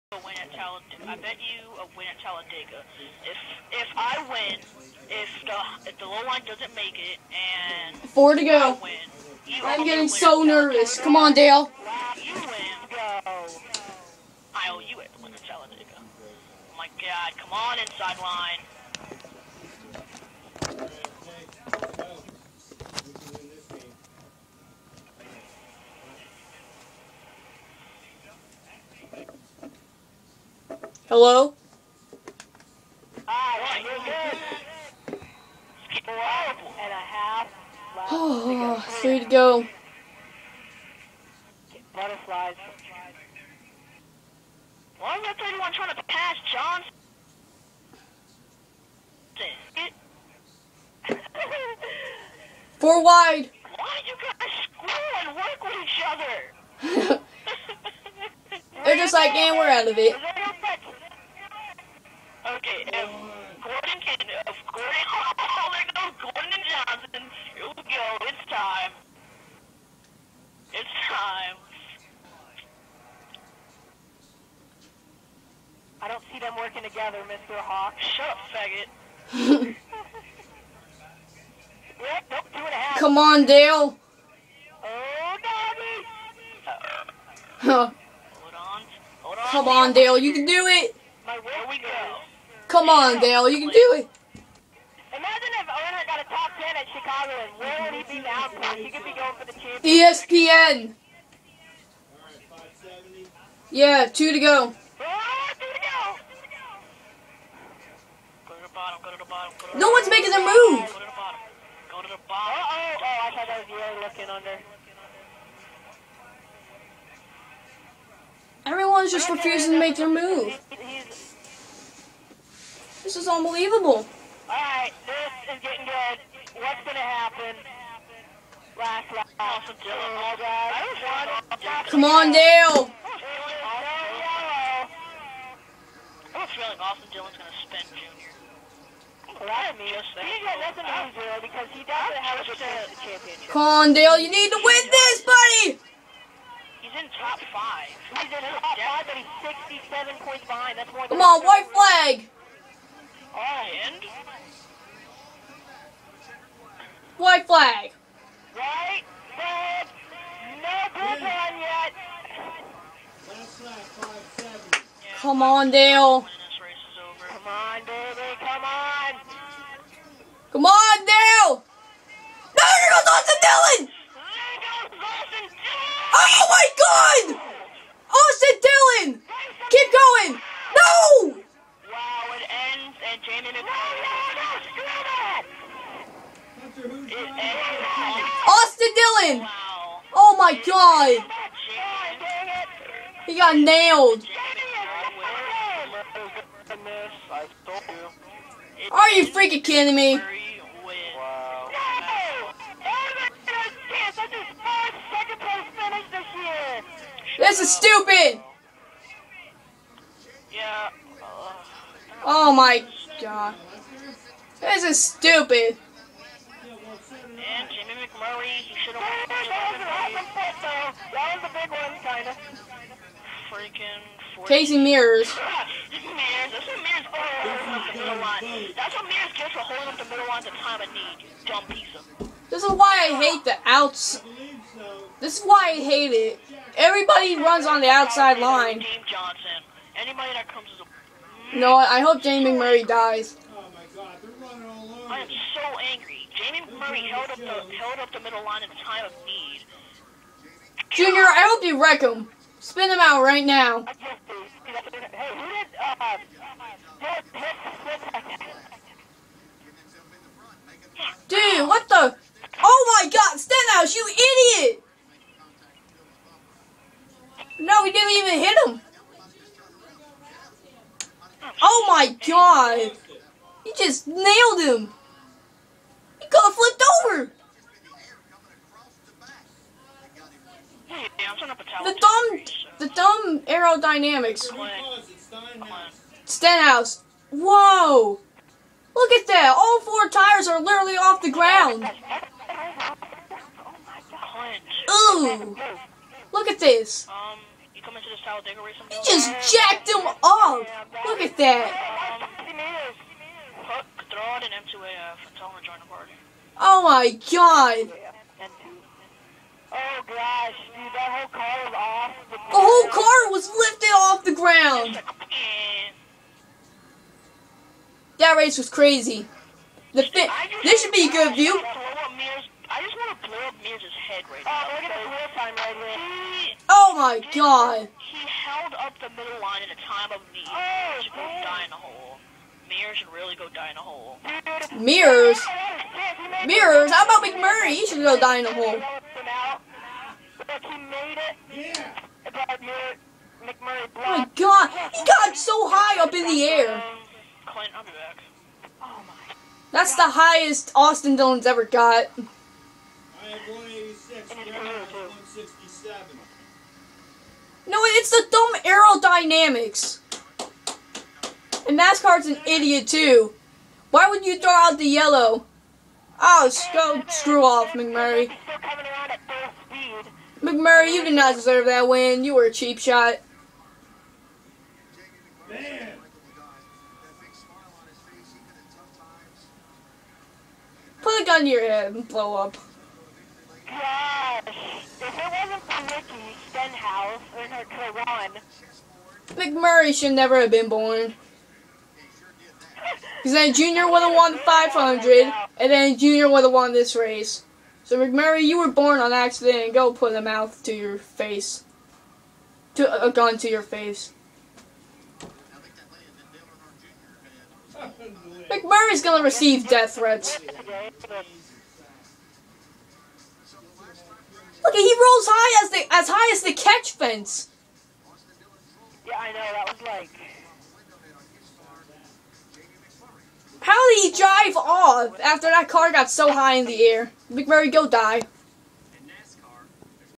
2013 NSCS Camping World RV Sales 500 Reactions ( Now In HD )
PEOPLE IN THE VIDEO - Clint8813, THE LONE HAWK48, TheProudUSMC, PxT Monaghan 99, & me DietDewHunter88.
0:00 - Coming 4 to go
2:52 - NOOOOOOOOOO AUSTIN DILLION
2:56 - OMG!
3:00 - NASCAR and its Cappy Cautions
4:26 - DUMB GEN-6 ARROW-DINAMICS